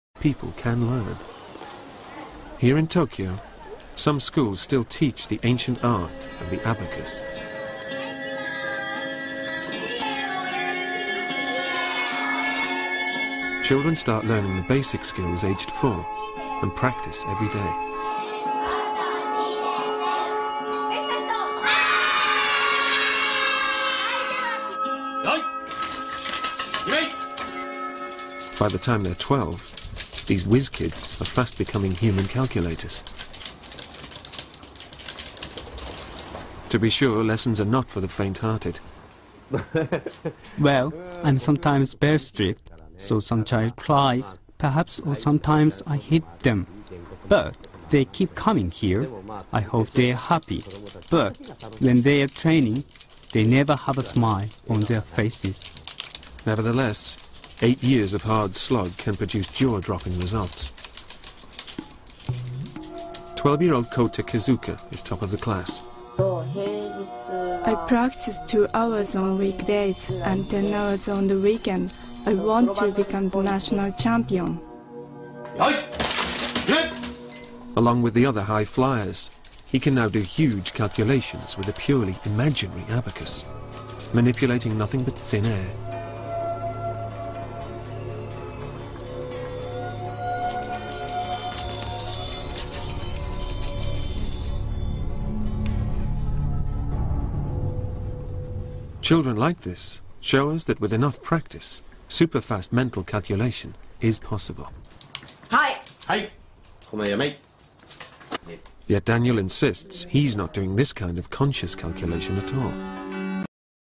Taller Abaco.avi
Este Verano Escolar 2011, desarrollaremos el taller ábaco, ya que con esto se desarrollo las matemáticas desde sus inicios y permite que los alumnos aprenda a razonar y analizar que es lo mas importante.